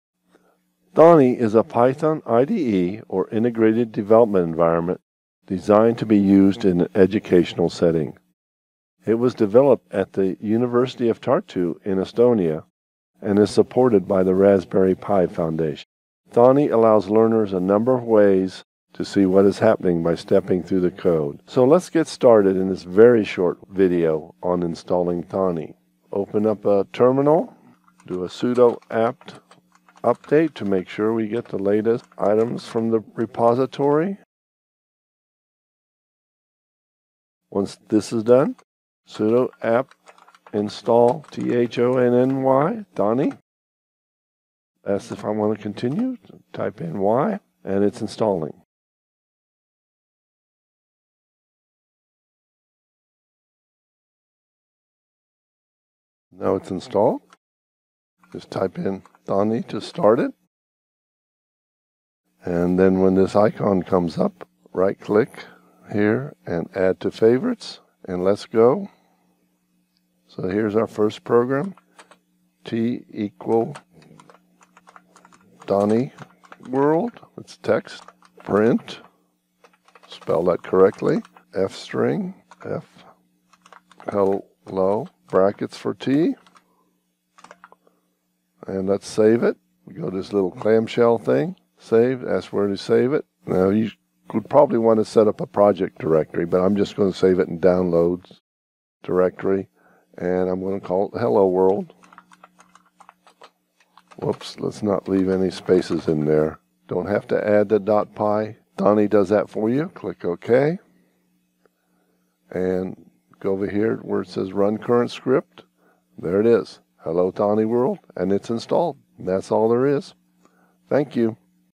Install Thonny Python IDE (Integrated Development Environment) into Ubuntu 20.04
Python in my professional opinion is the best first programming language to learn. New learners have a difficult enough time learning a programming language without having to struggle through a complicated text editor. Thonny is a Python IDE (Integrated Development Environment) especially designed for students to learn Python. This short video demonstrates how to install Thonny into Ubuntu 20.04.

A viewer asked how to install Thonny 4.01. Ubuntu does not have the latest version of Thonny in its repository so this video does not have the latest info. A newer version is available at:

0:00 Install Thonny
1:09 Hello Thonny World